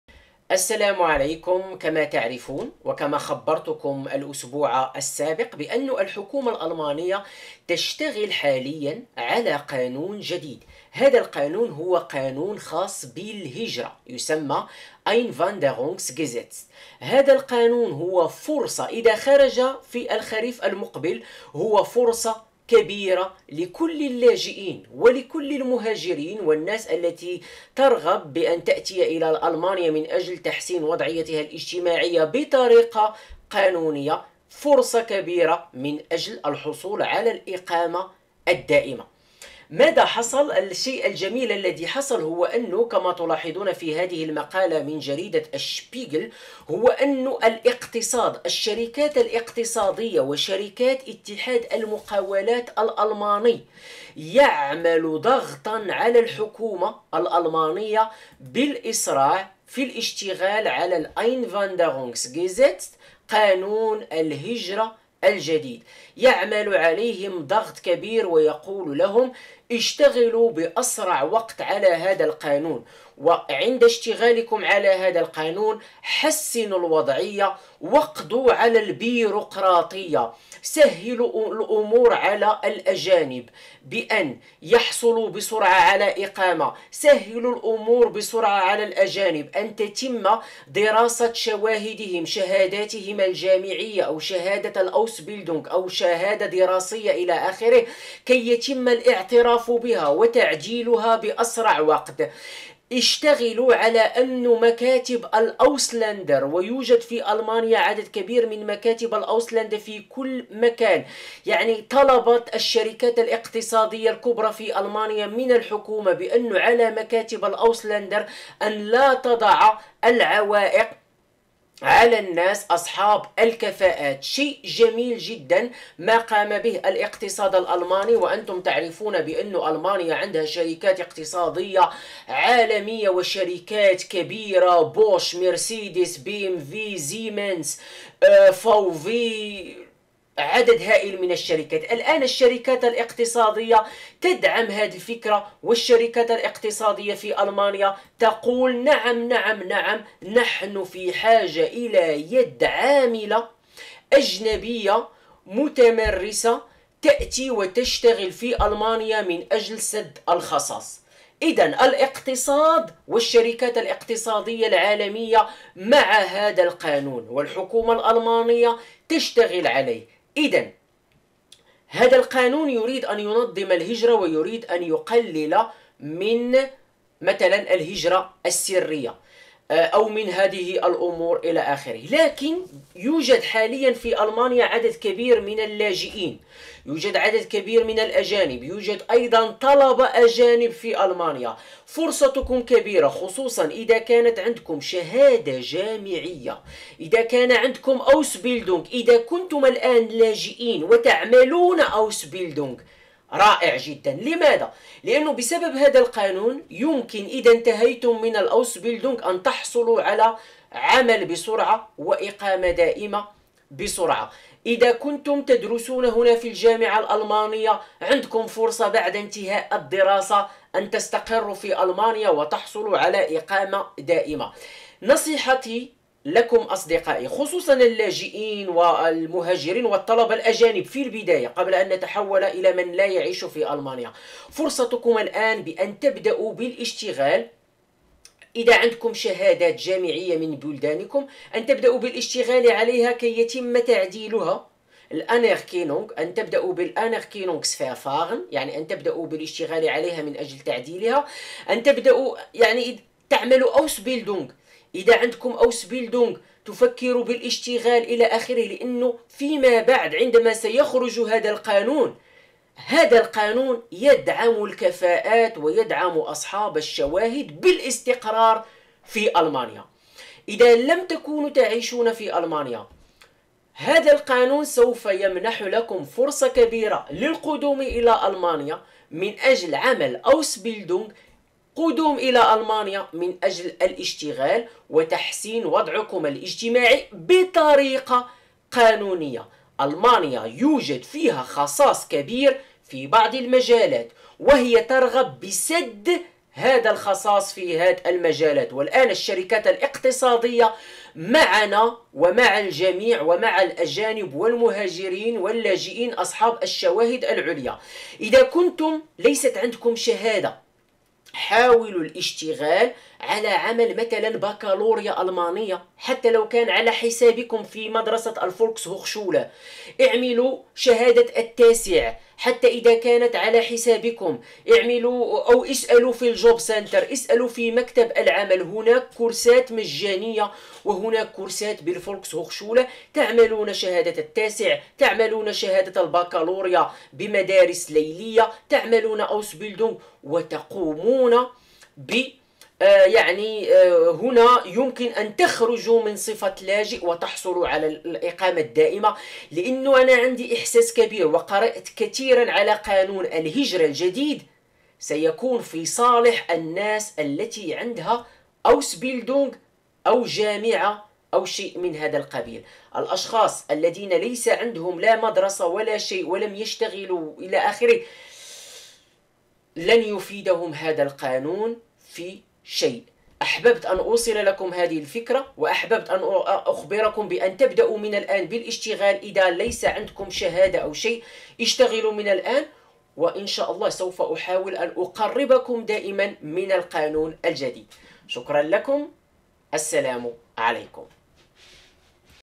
قانون الهجرة الجديد Einwanderungsgesetz فرصة اللاجئين و المهاجرين للحصول على الإقامة الدائمة
سوف أوافيكم بكل جديد حلو القانون إن شاء الله
المصدر
تعلم كيف تغير عقد شركة الكهرباء و توفر 500 يورو كل سنة بسهولة
صمم ثيابك بنفسك من هنا